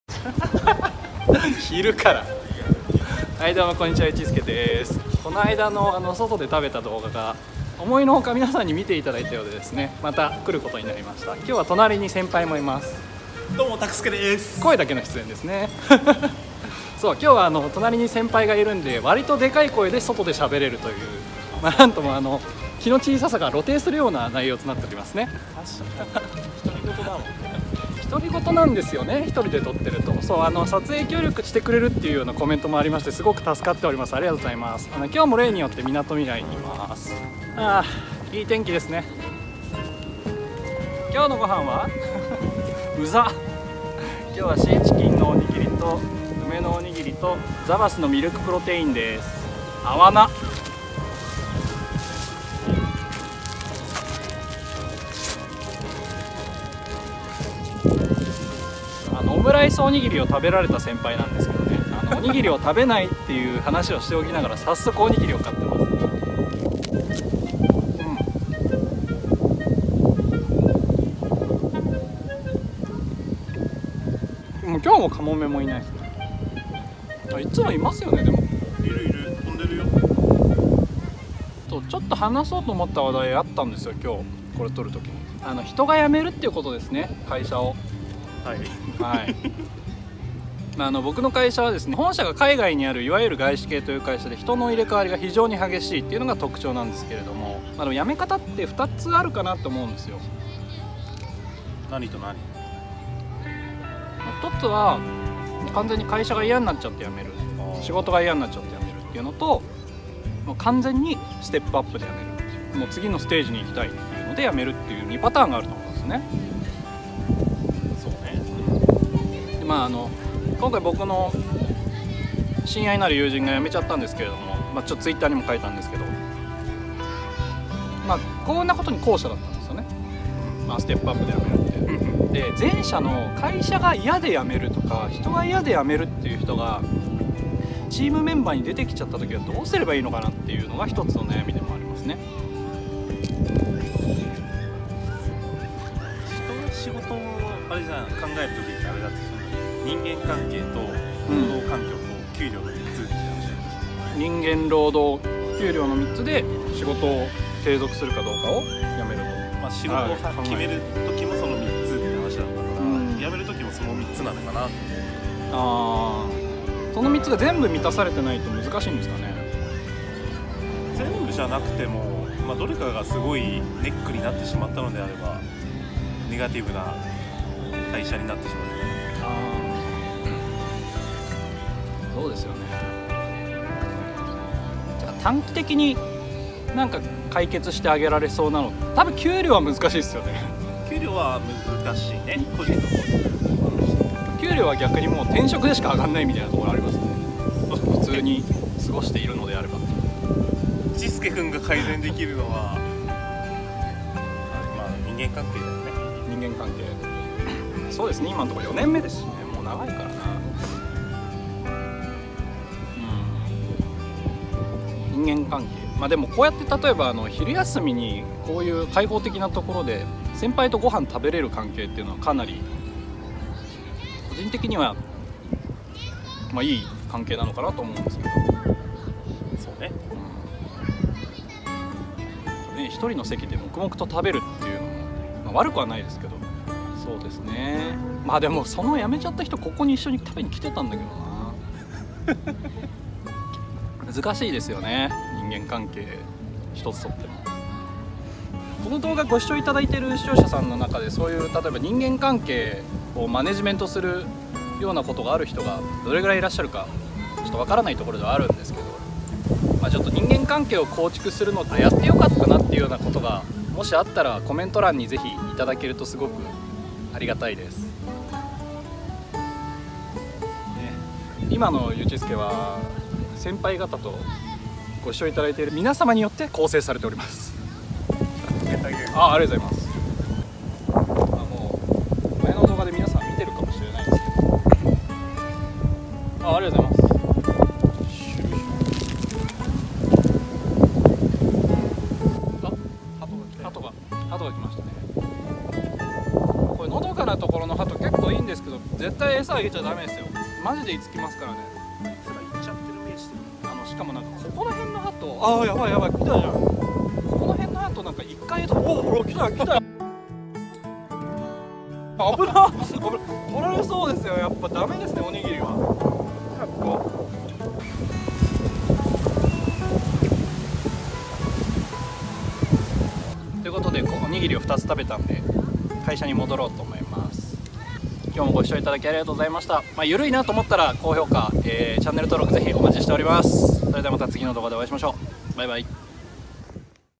【ランチルーティン】社会人営業職の日常ごはん【みなとみらいの日常】 - Lunch Time Routine
ランチの空き時間に片手間にご覧ください。やっぱりハトに襲われました。
ひとりごはんが多いので無理やり先輩を誘って撮影。

今日のごはんは何でしたか？
まったりごはんをつくって食べてます。

ひとりで食べるのがアレなので、
画面の向こうの誰かがなんとなくほっこりしてくれたらいいな。